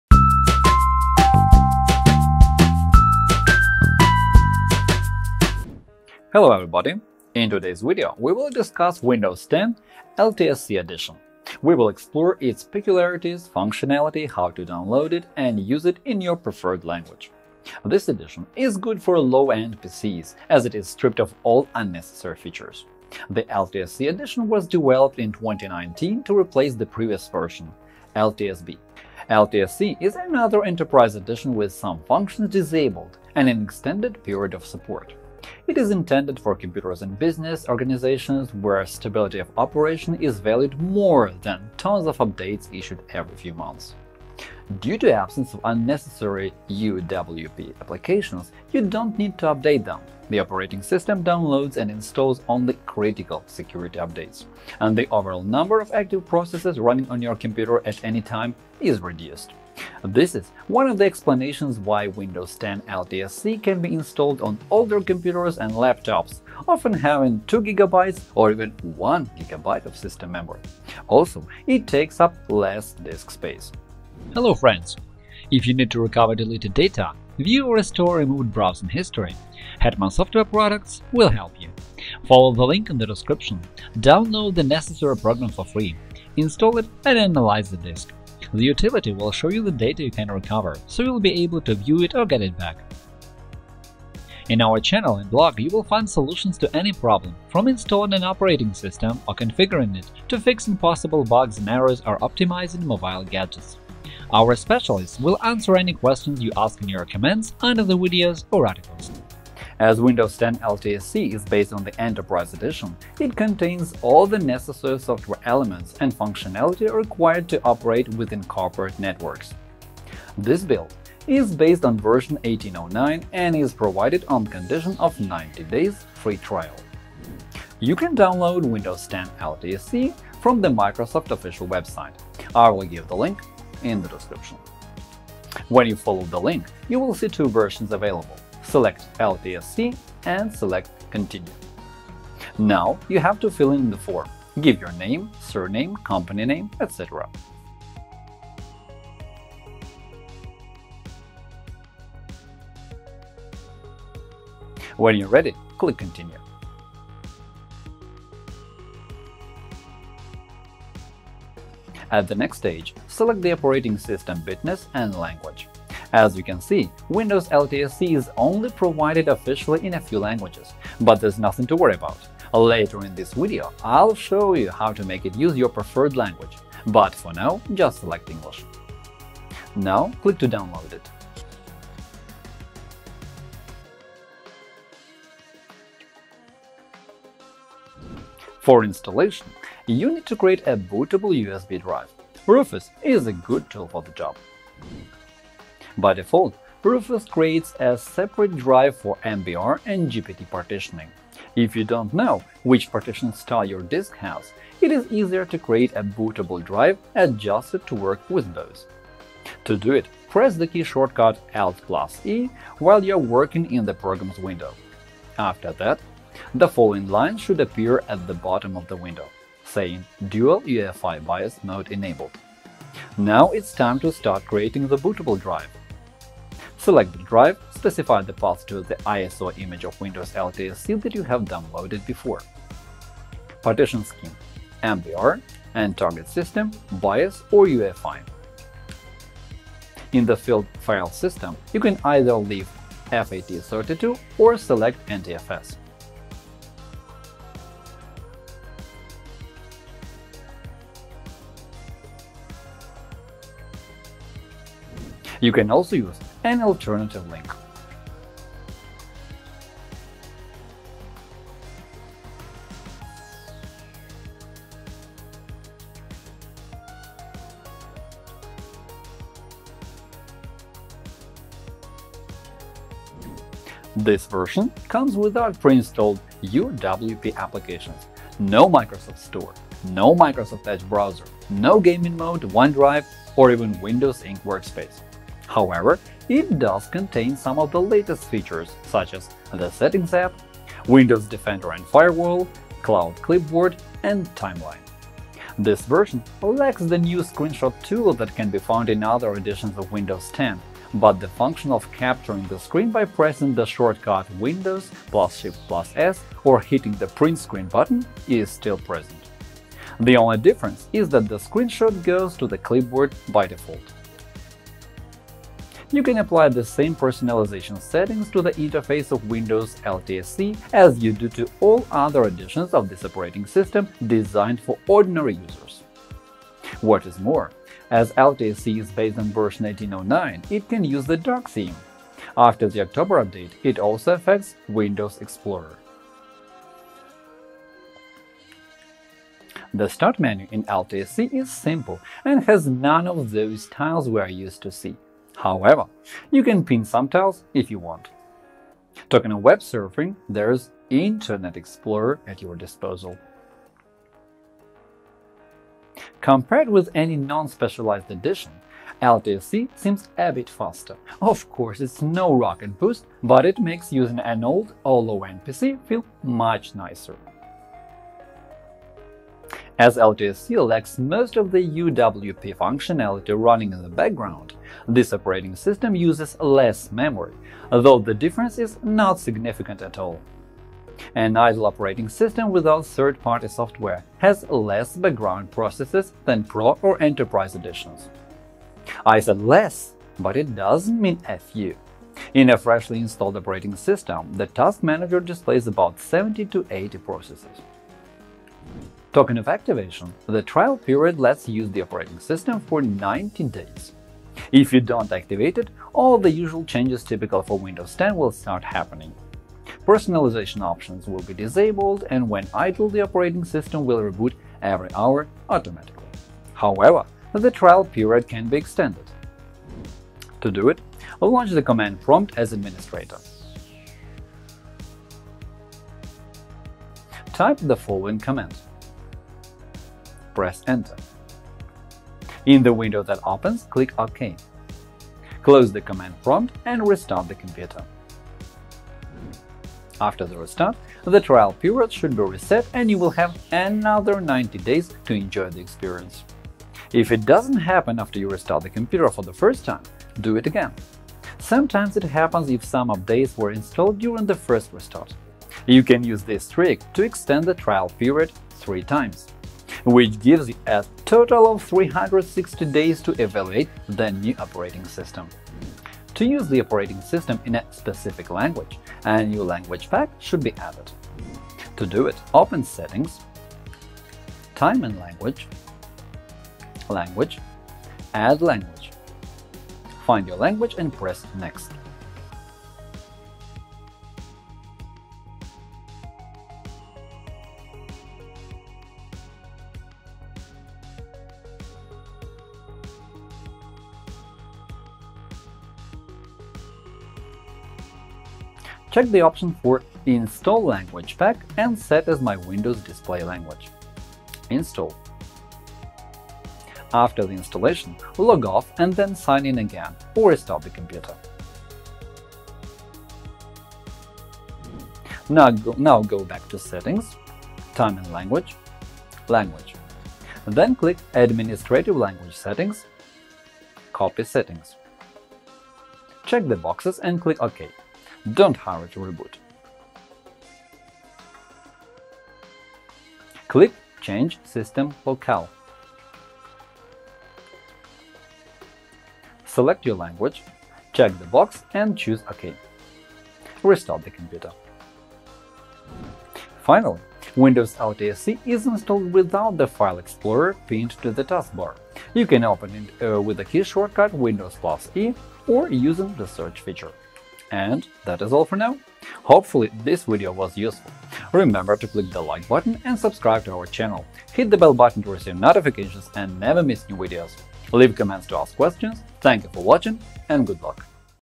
Windows 10 LTSC. The Fastest Operating System? 🚀🖥️
👍 In today’s video, we will discuss Windows 10 LTSC edition. We will explore its peculiarities, functionality, how to download it and use it in your preferred language. This edition is good for low-end PCS, as it is stripped of all unnecessary features.  The LTSC edition was developed in 2019 to replace the previous version, LTSB. LTSC is another Enterprise edition with some functions disabled and an extended period of support. It is intended for computers in business organizations where stability of operation is valued more than tons of updates issued every few months.

👉How to Find Out What Application Uses Microphone in Windows 10

📚 Content:
0:00 - How to download?
3:08 - How to create a bootable USB drive with MBR and GPT?
4:33 - What is Windows 10 LTSC 2019?
6:59 - How to extend the 90 days’ trial period?
7:45 - How to start using Windows 10 LTSC 2019 in your native language?

How to extend the 90 days’ trial period?
To do it, launch the Command prompt as Administrator. Type the following command:
slmgr –rearm
Enter. In the window that opens, click OK.
Close the command prompt and restart the computer.
After the restart, the trial period should be reset and you will have another 90 days to enjoy the experience.

Converting MBR into GPT and Vice Versa: How to Install Windows on GPT or MBR Drive

How to Install Windows 10 on a Computer or Laptop From a USB Drive or DVD

That is all for now! Hopefully, this video was useful. Leave comments to ask questions. Good luck.